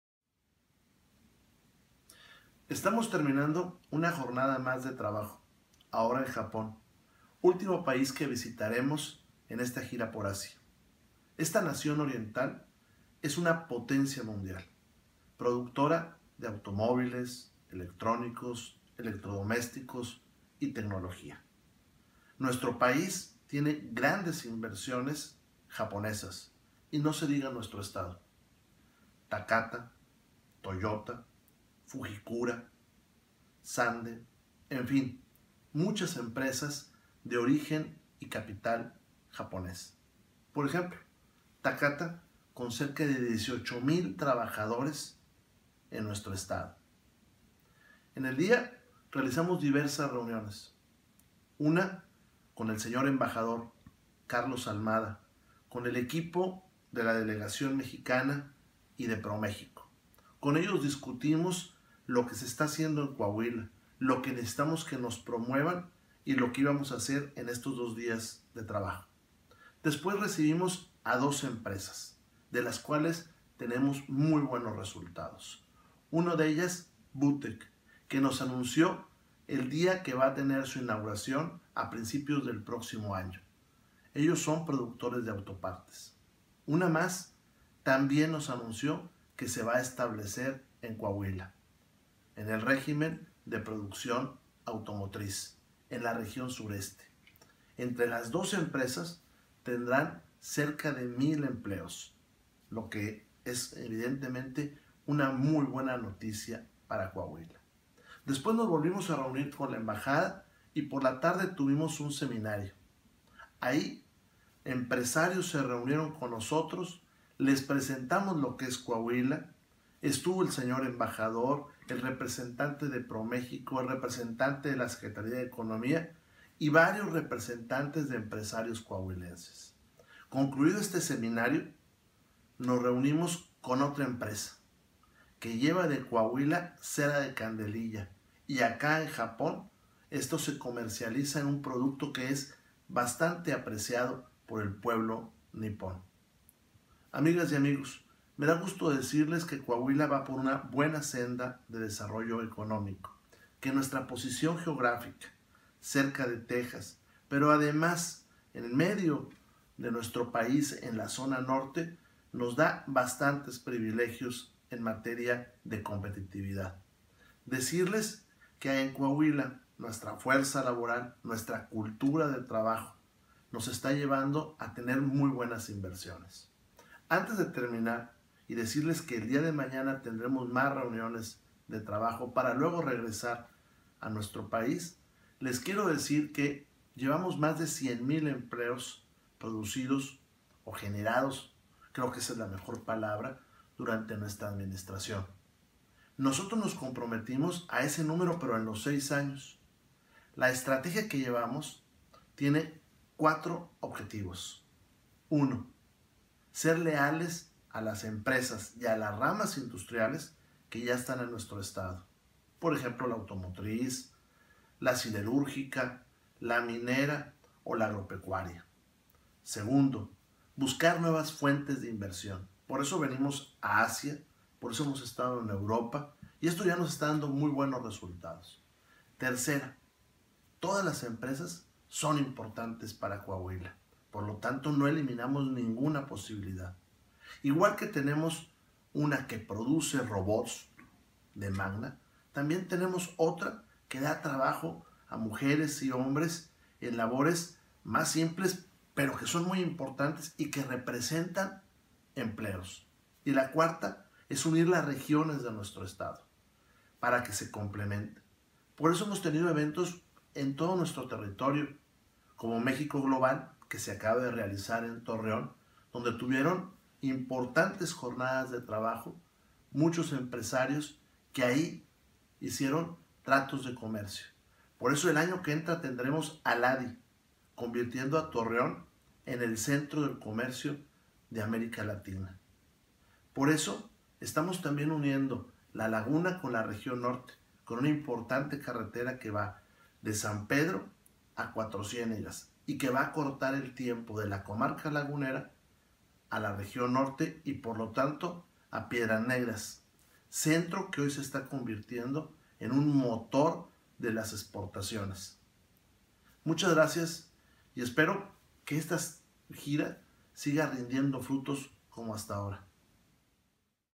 Gira Asia (Japón)
Seguimos promocionando a nuestro Estado por Asia.

Coahuila un Estado con Energía